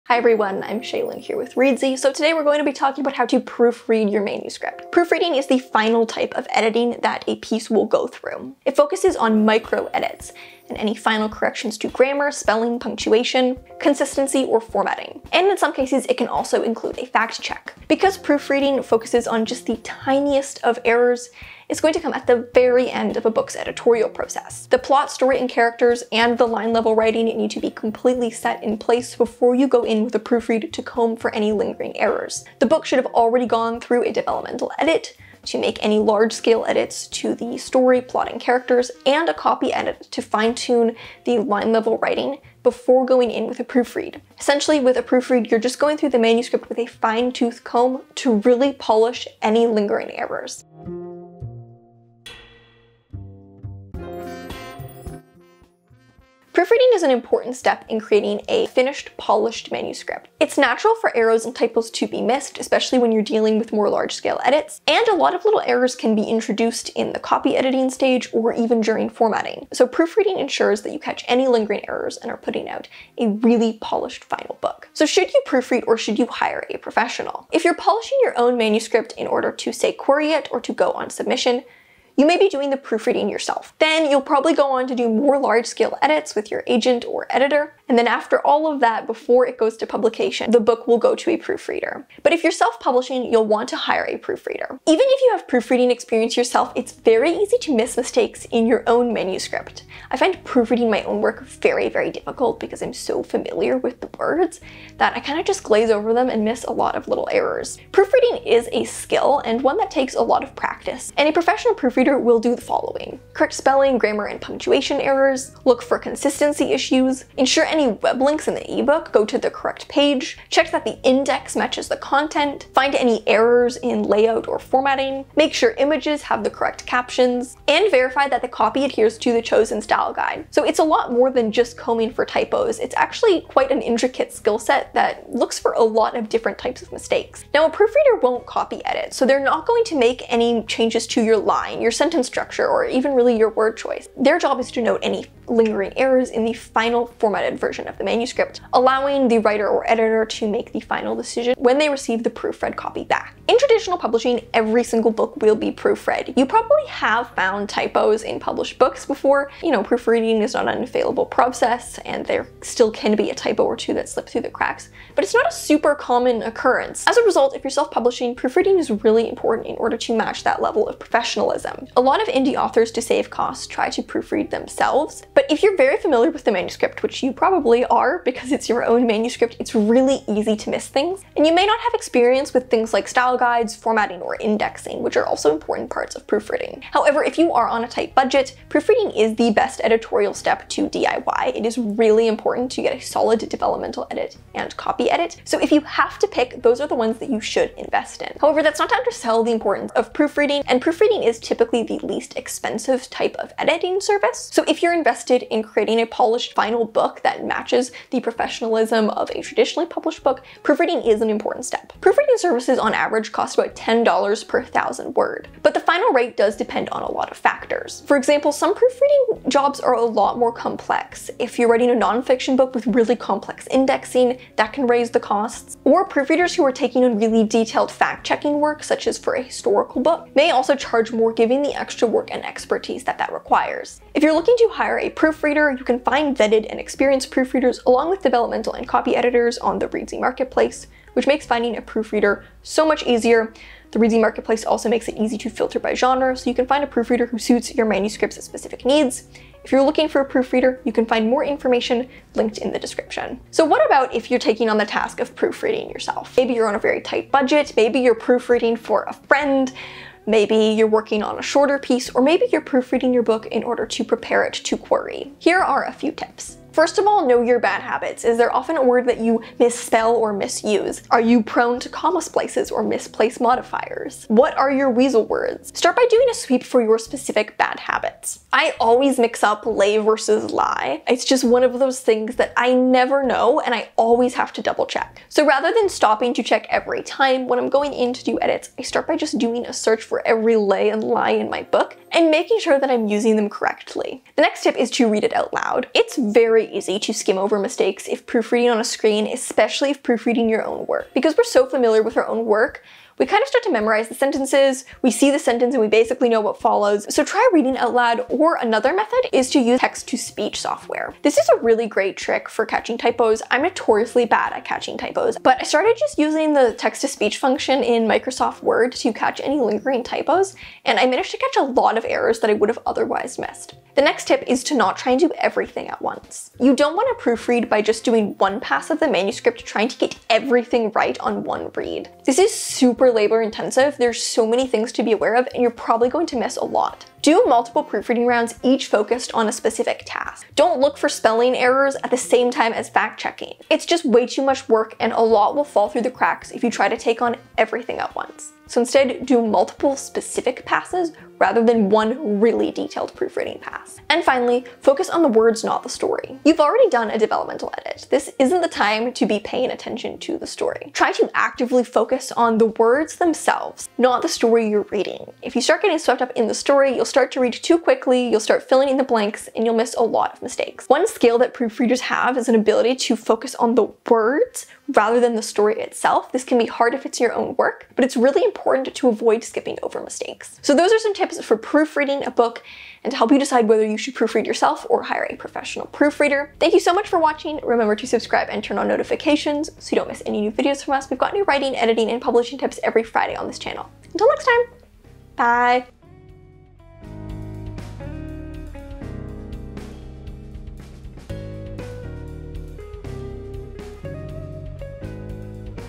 How to Proofread Your Manuscript
Proofreading is the final stage in editing a book, and makes sure that the manuscript is polished and ready for publication. In today's video, let's look at what proofreading is, whether you should hire a professional or do it yourself, and a few tips for more effective proofreading.

TIMESTAMPS:
0:00 - What is proofreading?
1:15 - Should you proofread yourself?
4:15 - Know your bad habits
4:55 - Read the book out loud
5:22 - Proofread in passes
5:55 - Focus on the words, not the story